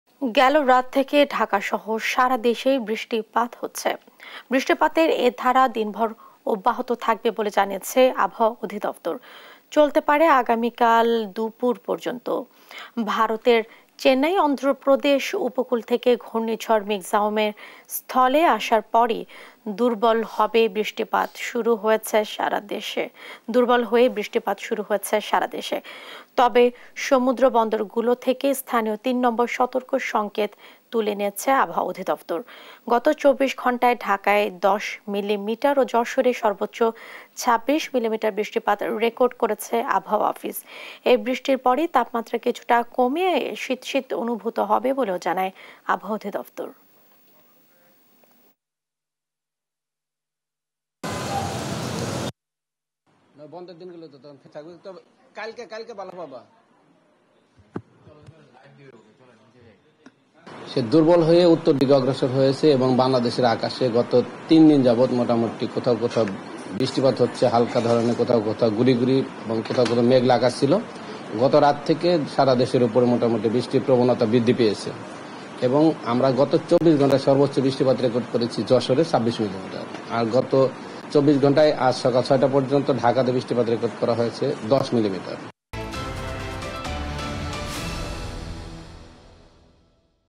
বৃষ্টির ধারা অব্যাহত থাকবে কয়দিন জানাল আবহাওয়া অফিস | Weather Update | Desh TV
»» Office Address:
42, Shaheed Sangbadik Selina Parveen Sarak, Malibag 1217 Dhaka, Dhaka Division, Bangladesh